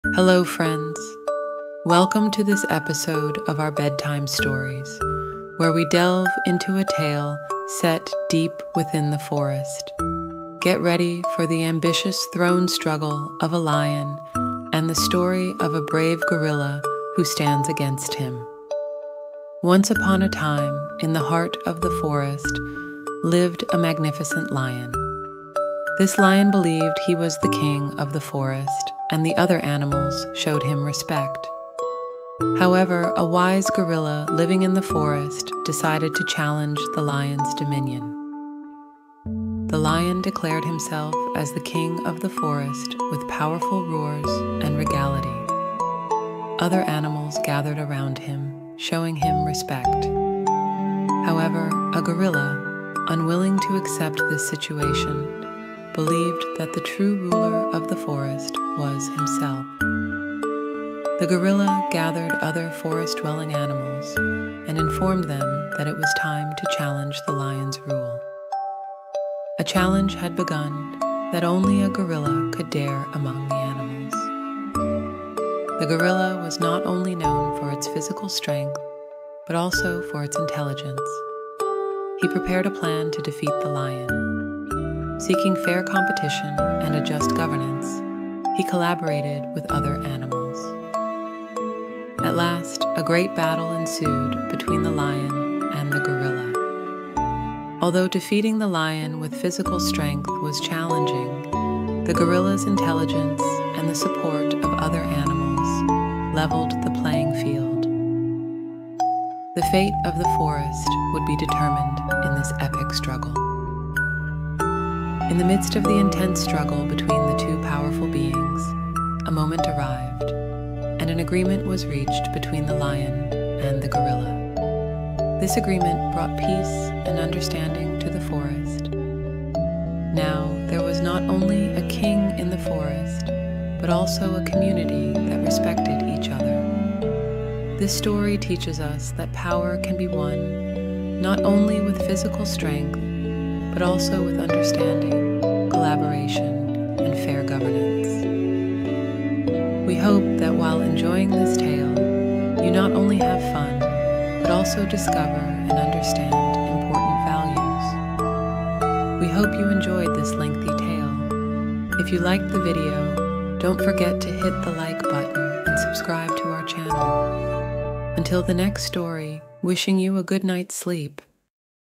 Ruler of the Forest: Lion or Gorilla?
Before sleep children's tales.
Who is the king of the forest, the lion or the gorilla?